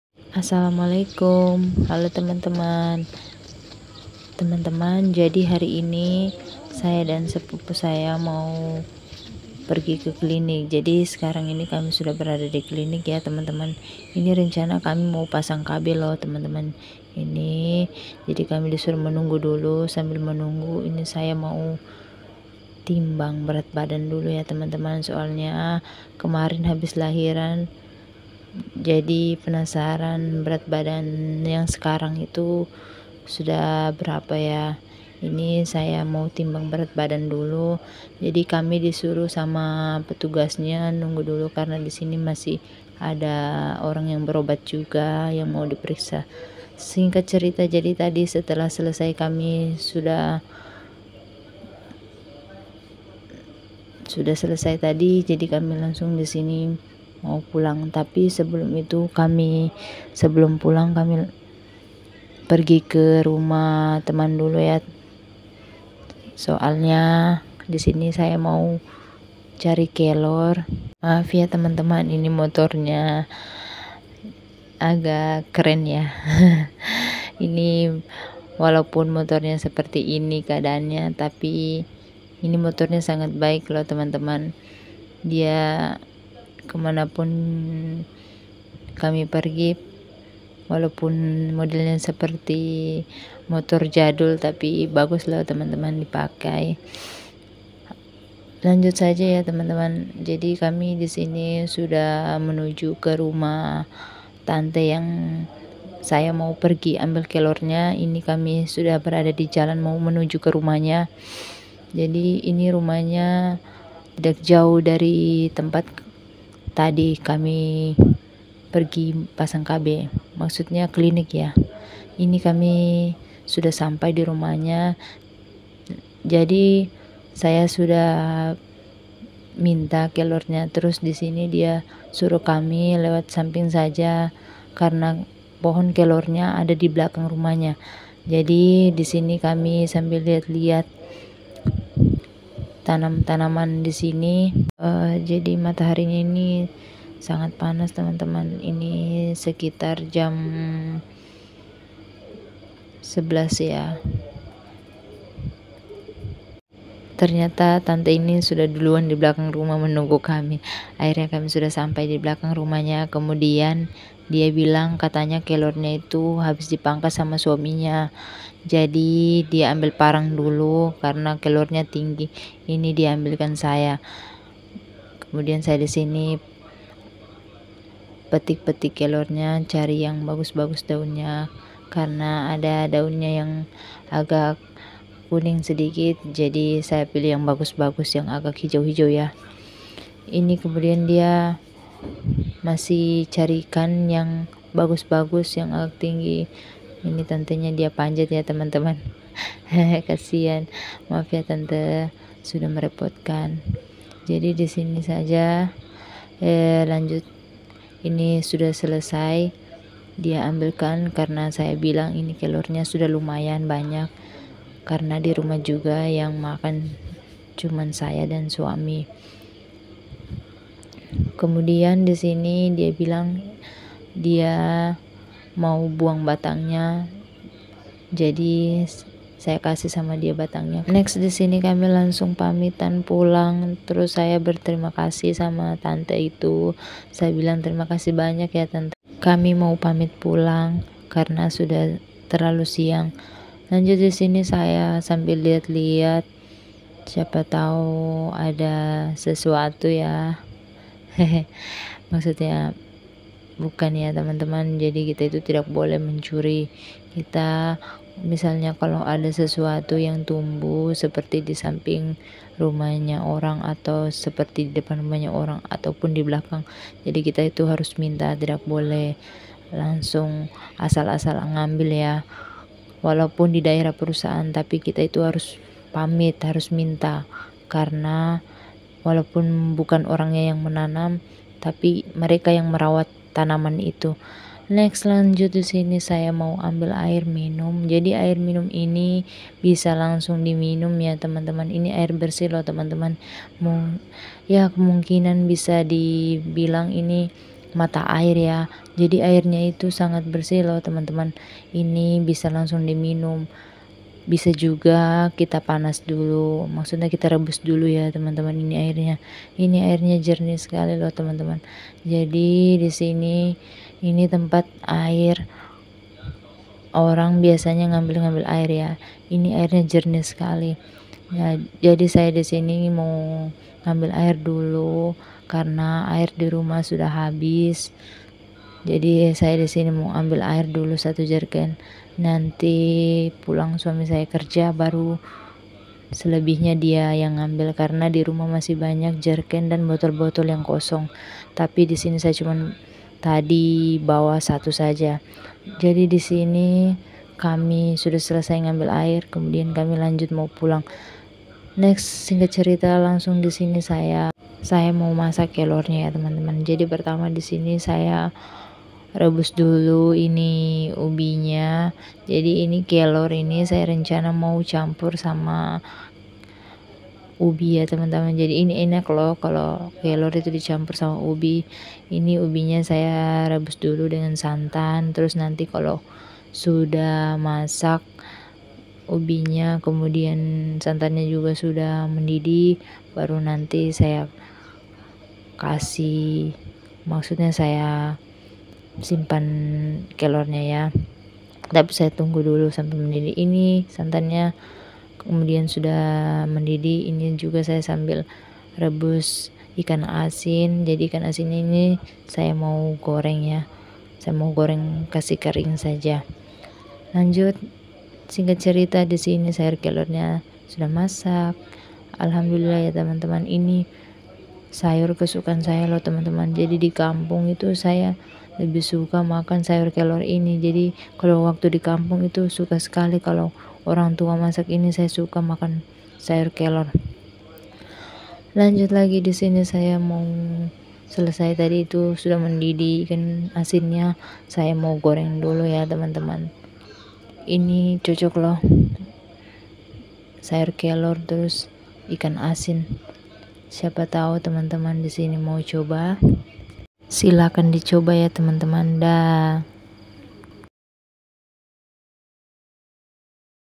PERGI KLINIK LANJUT CARI KELOR DAN MASAK SAYUR KELOR
Assalamualaikum

Halo teman teman, selamat datang di channel Sukma Ayu bagaimana kabarnya hari ini?
 Dimana pun kalian berada semoga kita selalu diberikan kesehatan kebahagian dan dilancarkan rezeki kita semua Aminn 🤲
Aktivitas hari ini pergi klinik lanjut cari kelor
resep :
Kelor
santan
air
Lombok